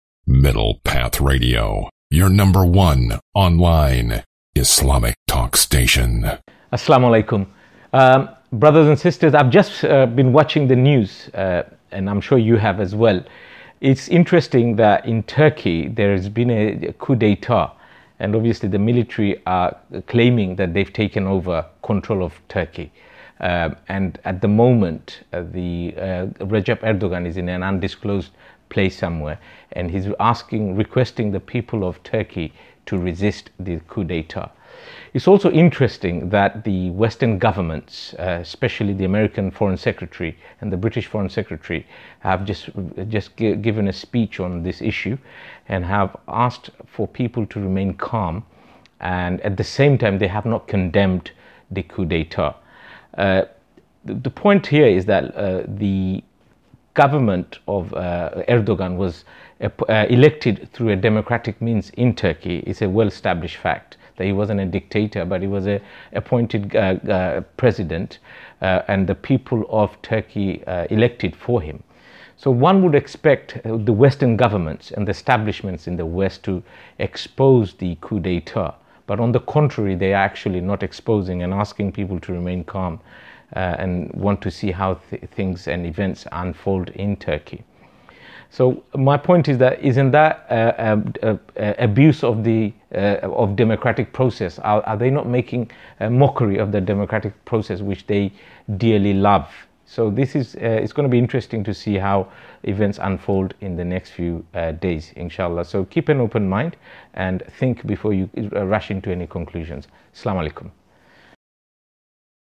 Coup in Turkey
Initial reaction from Muslims in UK.

Kuddus Ali - Straight Talk presenter